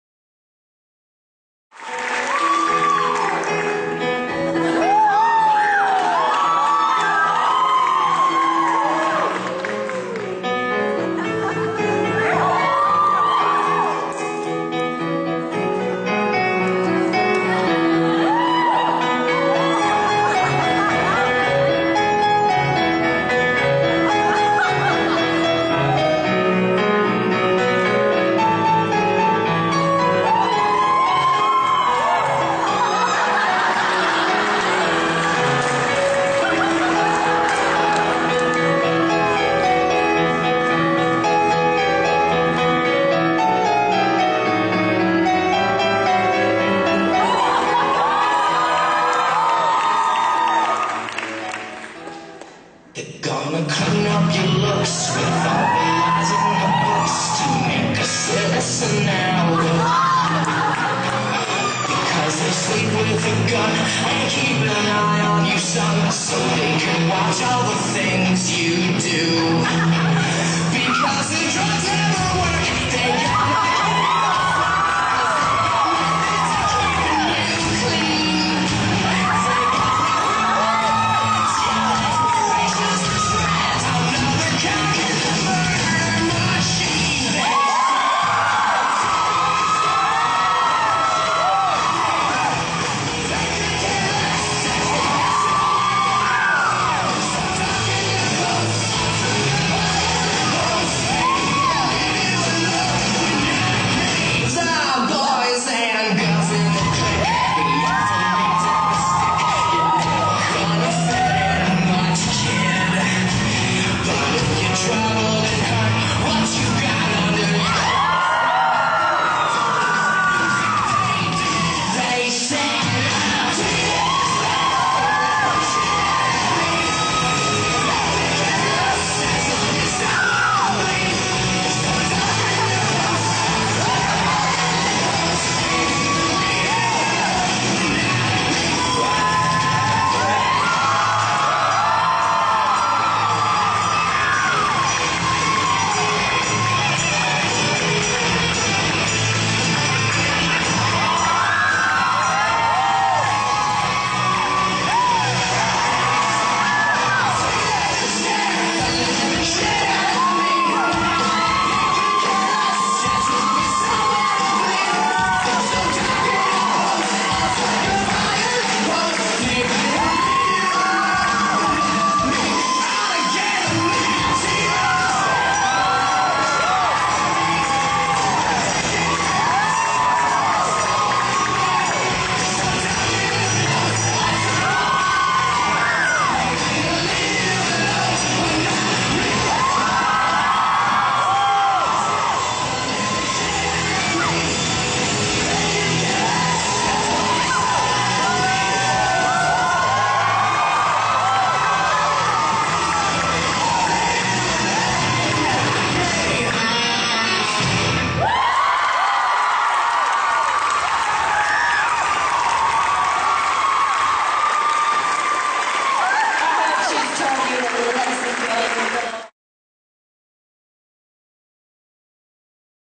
Miss Burlesque QLD 2013 Neo The Diamond Dahlia
Neo Burlesque Act by The Diamond Dahlia "Teenage Etiquette"
Filmed by MBA at The Old Museum 2013